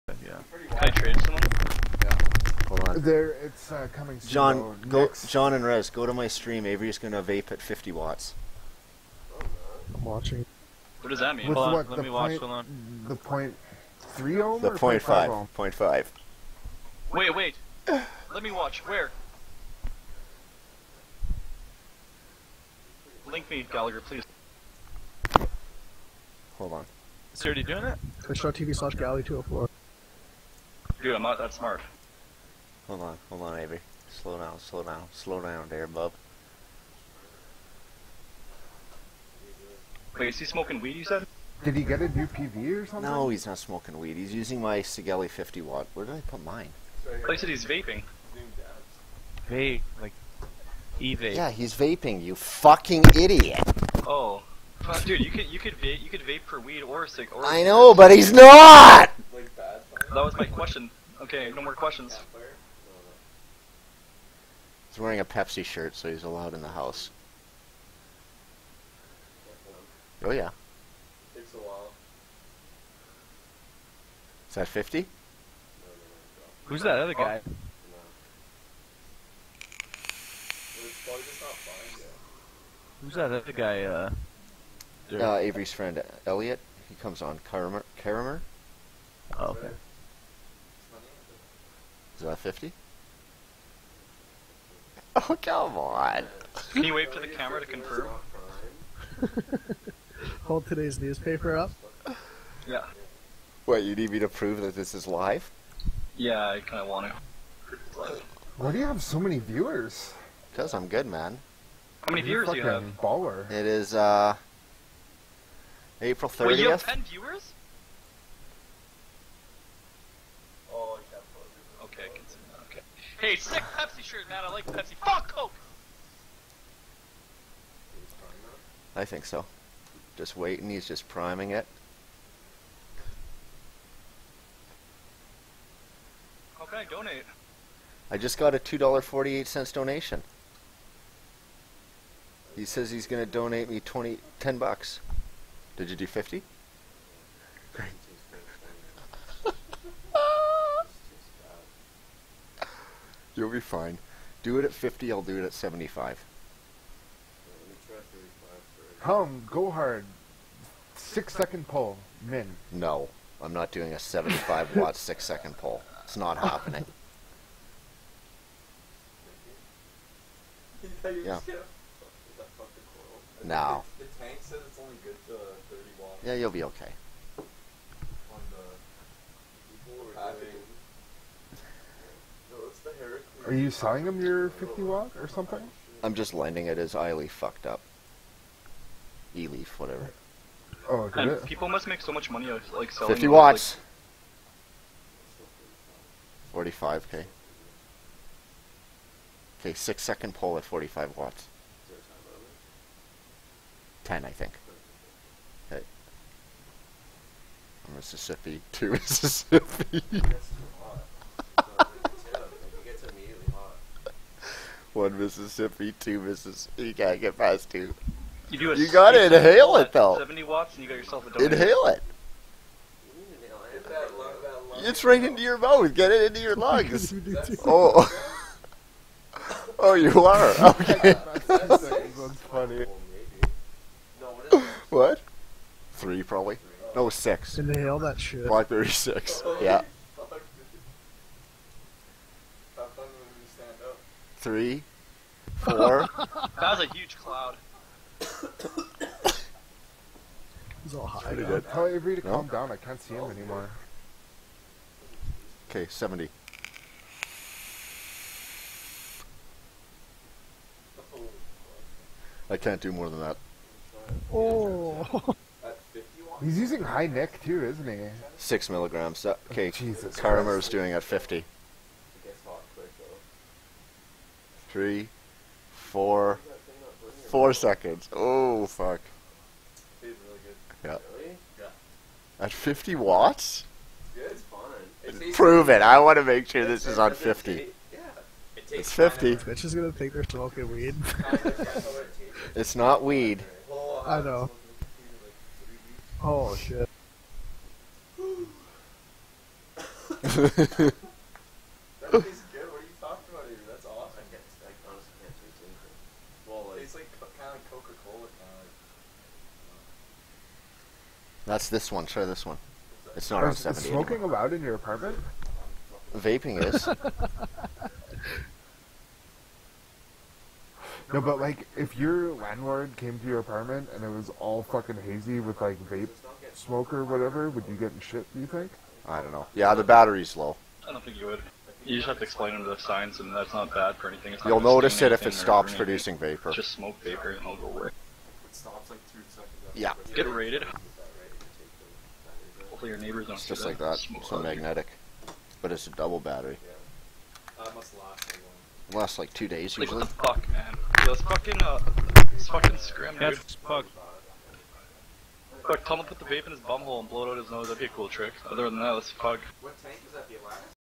Prismat1c over Vaping at 50Watts
Mr. Prismat1c over Vaping at 50Watts 6mg nicotine blackberry.